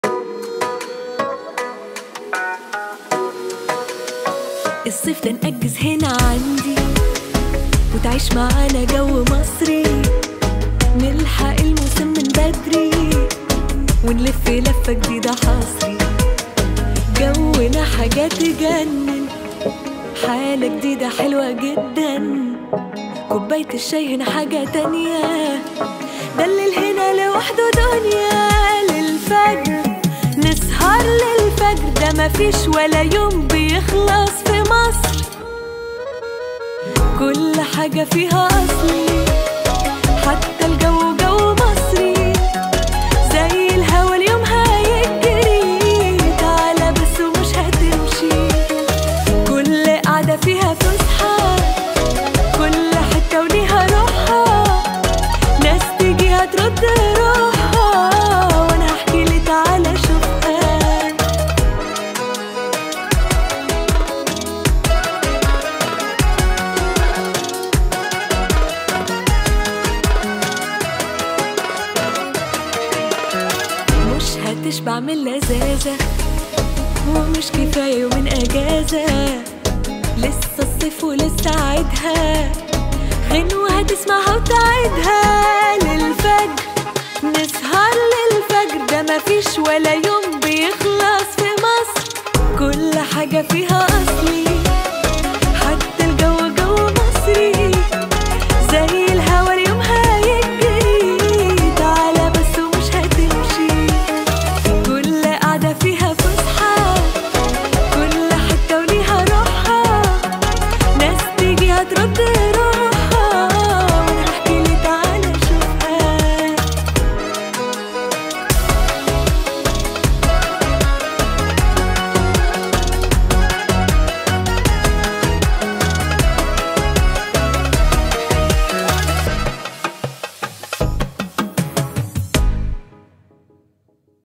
حملة ترويجية لمصر خلال الصيف
أطلقت وزارة السياحة والآثار ممثلة في الهيئة المصرية العامة للتنشيط السياحي، بدءاً من اليوم الخميس الموافق 30 يونيو 2022، وخلال أشهر الصيف، حملة ترويجية تحت عنوان "اليوم في مصر ما بيخلصش"، وذلك للترويج السياحي لمصر خلال موسم الصيف، وبالأخص في السوق العربي ومنطقة الخليج، بالإضافة إلى تنشيط السياحة الداخلية في مصر.